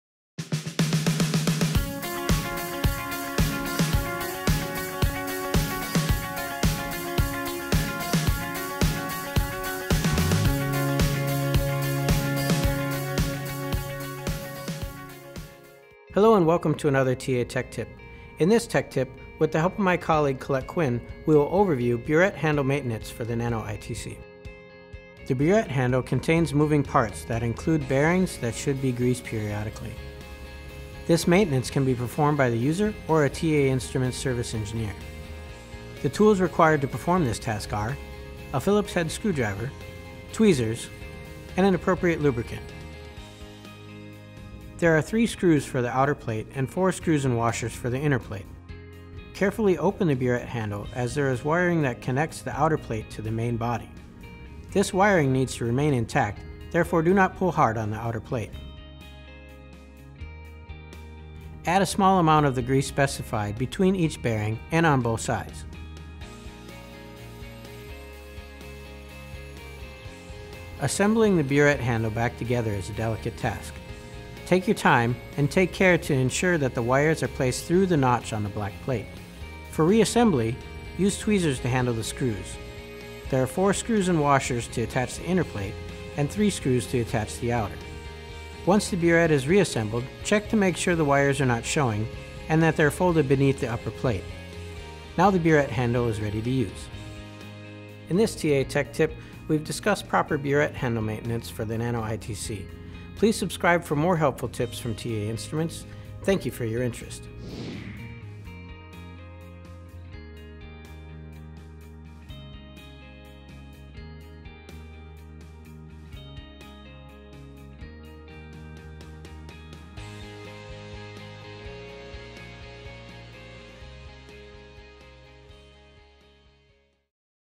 Buret Maintenance For Nano ITC
In this TA Tech Tip, we discuss proper maintenance steps for the Nano ITC buret.